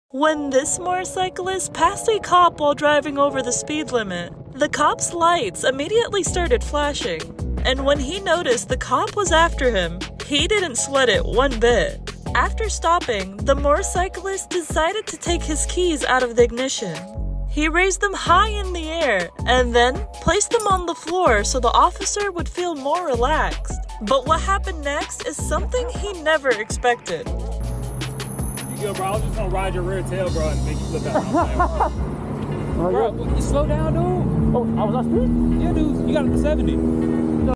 We Need More Cops Like This - Unexpected Traffic Stop ♥️
When this motorcyclist went over the speed limit while passing a cop, he ended up being pulled over, but what happened next turned out to be an unforgettable traffic stop by one of the coolest police officers ever! (wishesm3 YT)